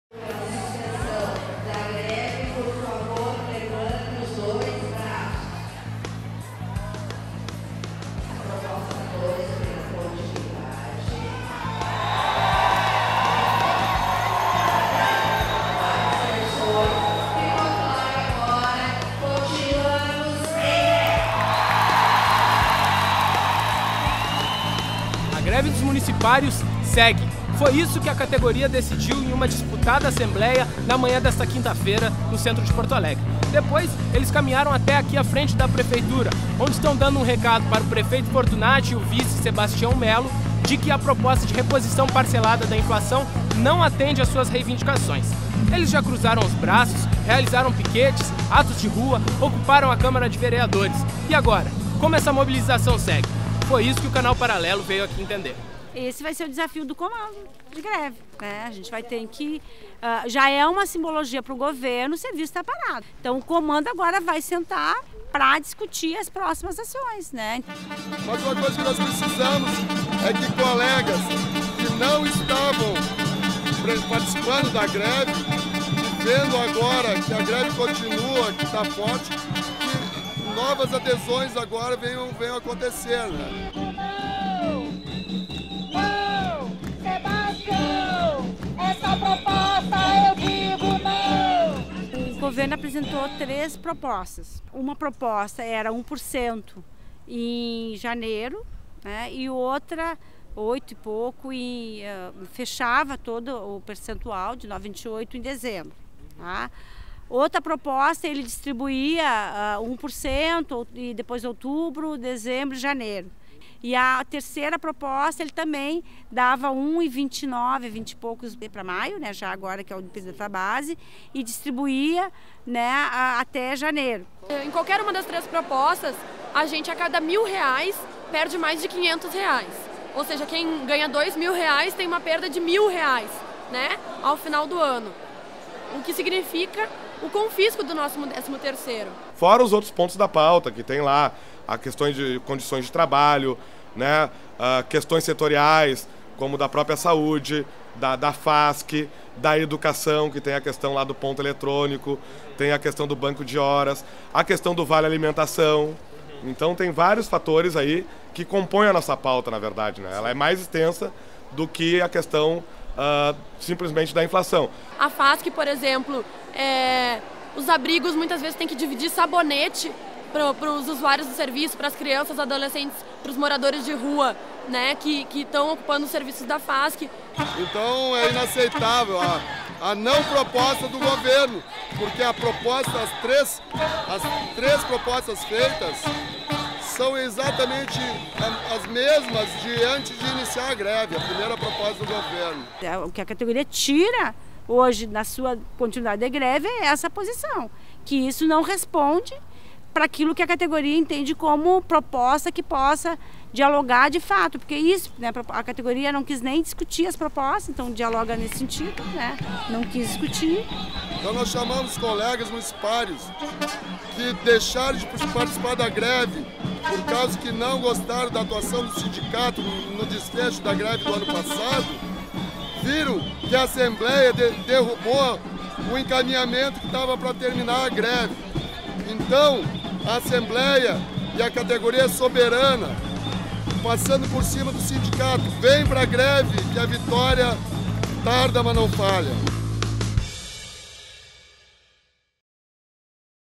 MUNICIPÁRIOS SEGUEM EM GREVE
SERVIDORES DO MUNICÍPIO REJEITAM PROPOSTA DA PREFEITURA E GREVE CONTINUA
Nova assebleia prevista para 28 de junho

Os municipários rejeitaram, na manhã de ontem, as três propostas de reajuste apresentadas pela Prefeitura. Em uma assembleia tensa na Casa do Gaúcho, a maioria dos servidores optou por seguir com a greve, alegando que todas as propostas representam perda salarial. Uma nova assembleia está programada para a próxima terça-feira, 28/06 às 14h, também na Casa do Gaúcho.

Confere o recado dos servidores!